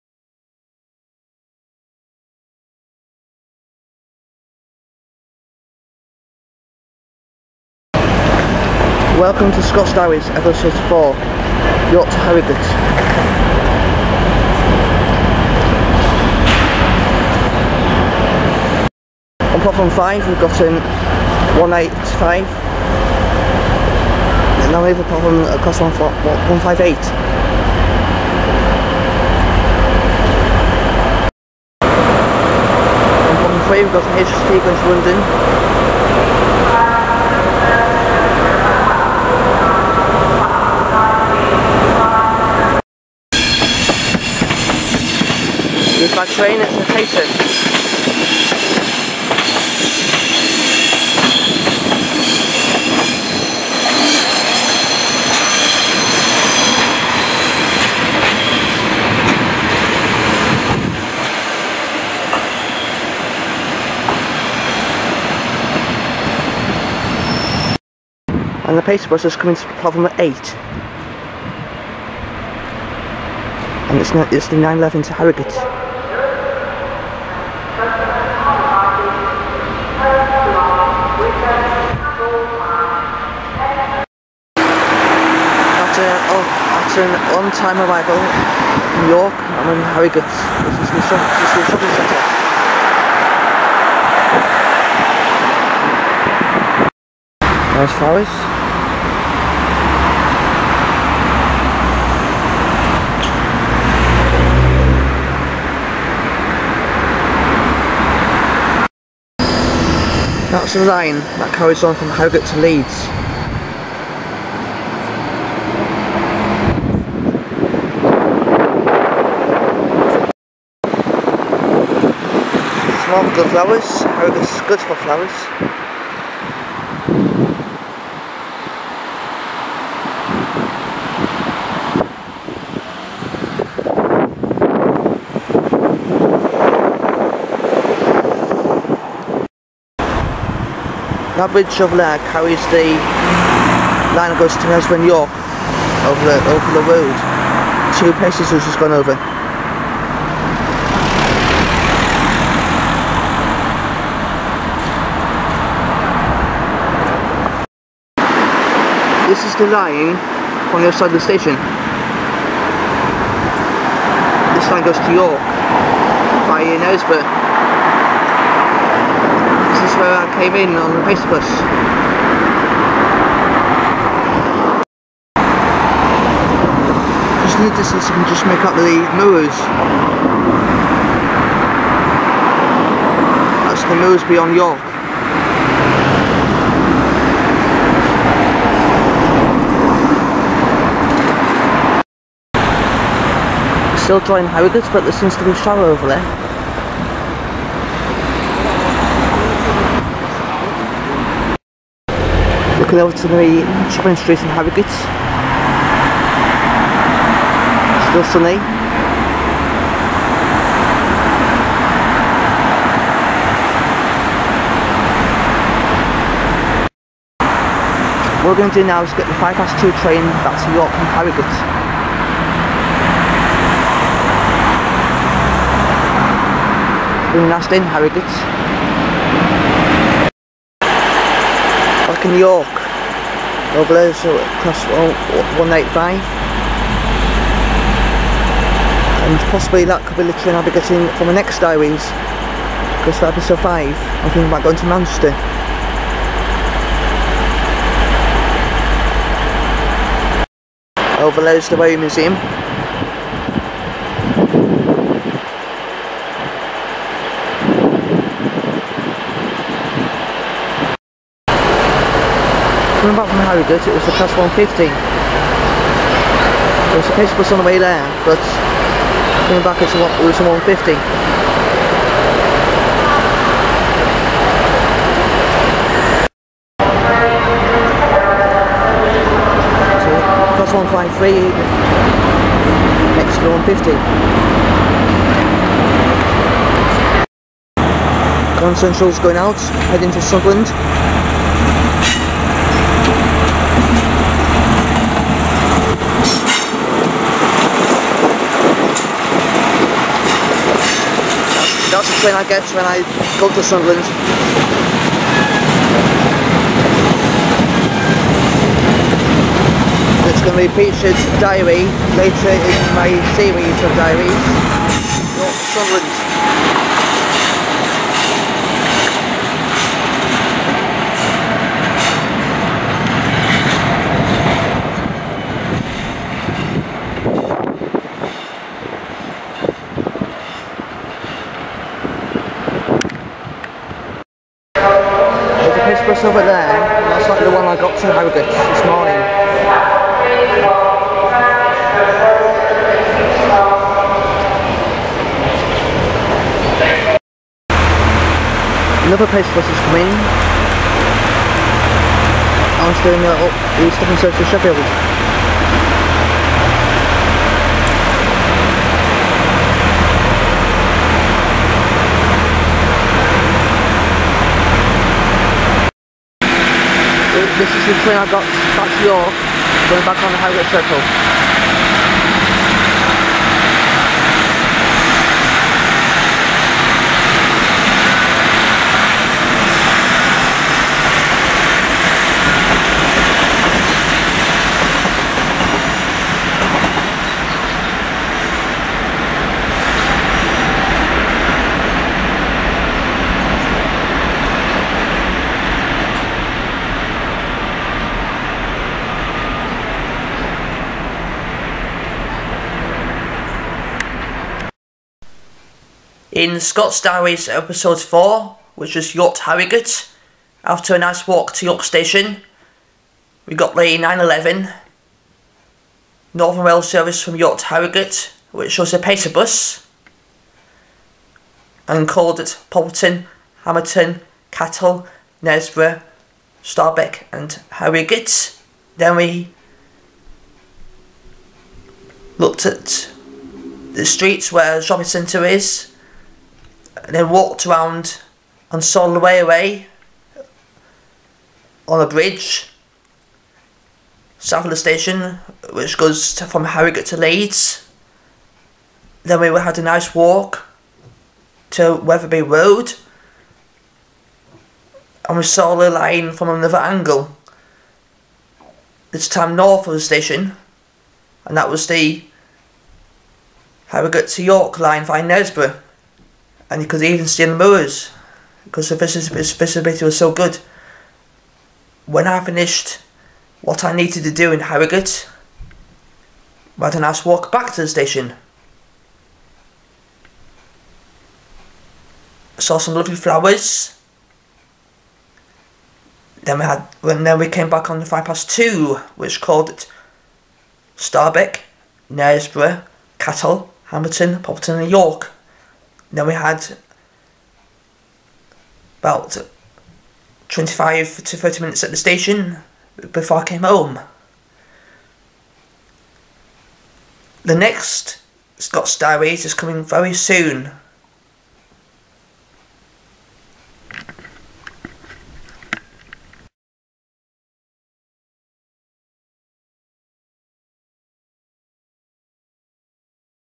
Scott's Diaries Episode 4 - York to Harrogate.
In Scott's Diaries Episode 4 I went on the train from York to Harrogate.  After a lovely walk to the Station I caught the 09:11 Northern Rail train which called at Poppleton Hammerton Cattal Knaresborough Starbeck and Harrogate.  In Harrogate I have a walk around and saw some lovely flowers.

After the meeting I had a lovely walk before catching the 14:05 train back to York which called at Starbeck Knaresborough Cattal Hammerton and Poppleton.  I then had a look at the trains in York Station before going home.

My Twitter is now @ScottMike444.